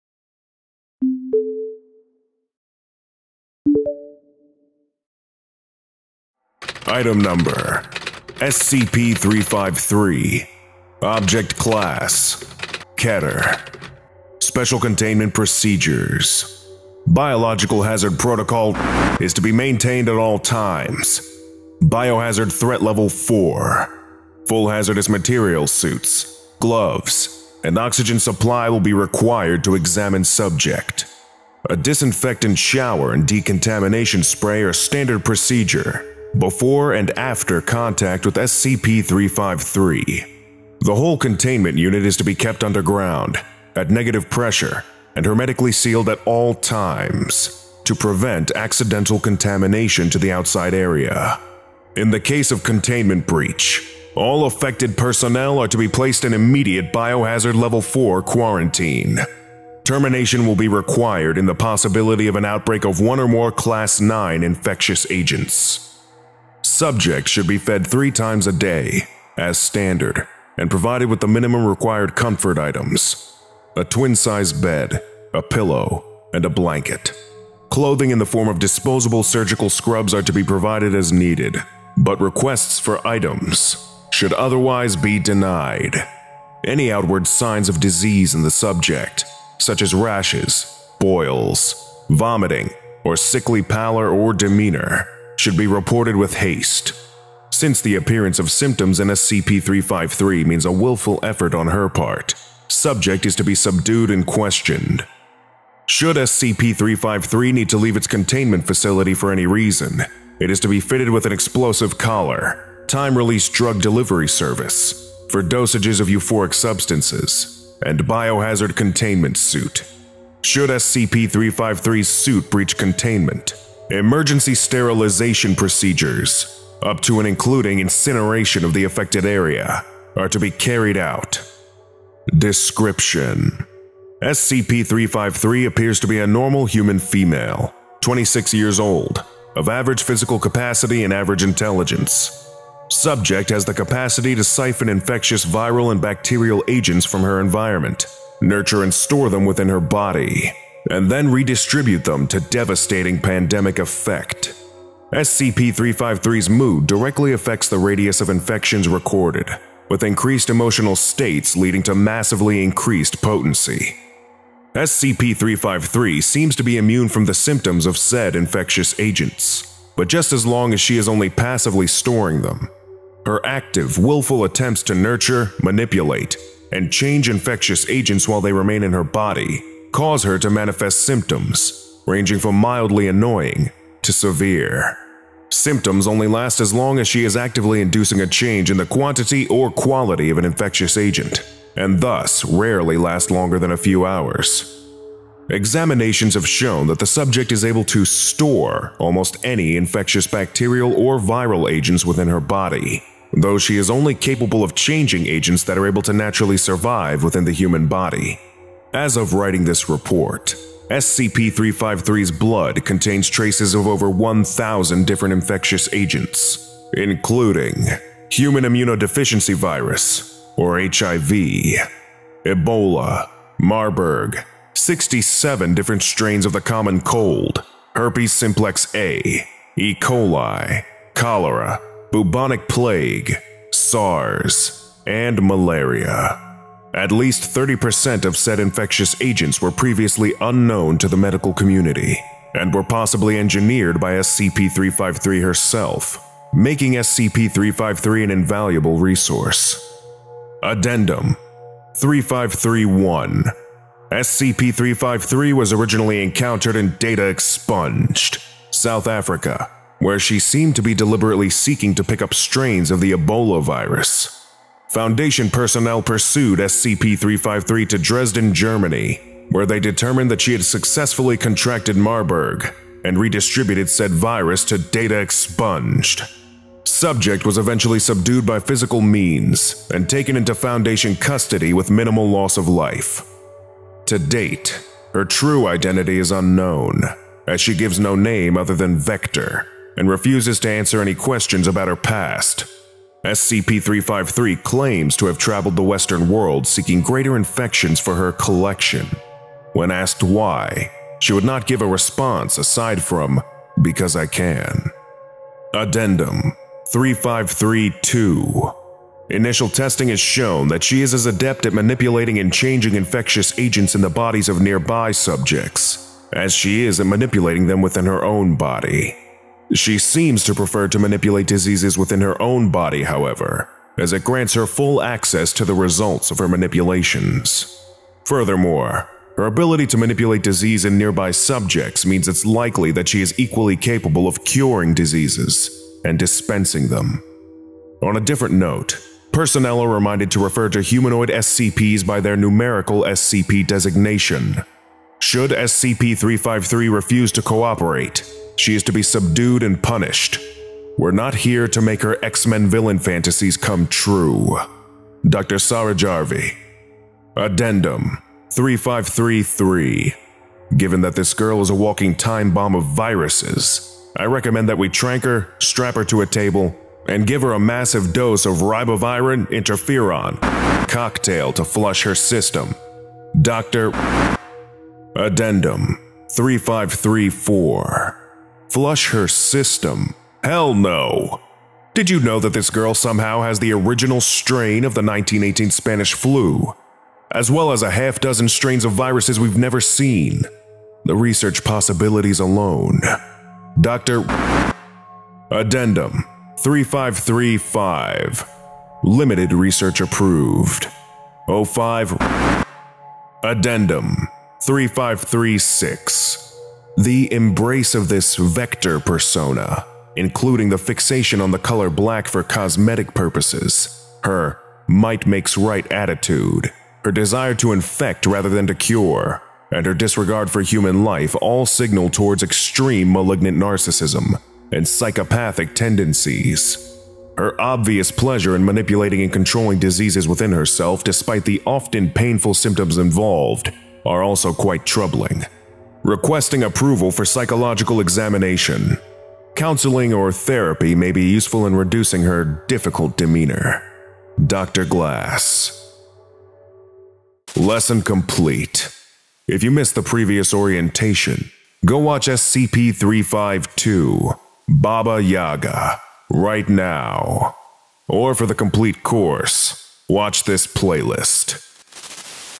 SCP-353 | Vector (SCP Orientation)
Today we will be studying Item number SCP-353: Vector, Object class: Keter.

SCP-353 appears to be a normal human female, twenty-six years old, of average physical capacity and average intelligence. Subject has the capacity to siphon infectious viral and bacterial agents from her environment, nurture and store them within her body, and then redistribute them to devastating pandemic effect. SCP-353’s mood directly affects the radius of infections recorded, with increased emotional states leading to massively increased potency.

To date, Subject’s true identity is unknown, as she gives no name other than "Vector" and refuses to answer any questions about her past. SCP-353 claims to have traveled the western world seeking greater infections for her "collection". When asked why, she would not give a response aside from, "Because I can."

and released under Creative Commons Sharealike 3.0. Contributors: Pair Of Ducks (rewrite author), Slate (author)

Attribution:  National Museum of Health and Medicine, Armed Forces Institute of Pathology, Washington, D.C.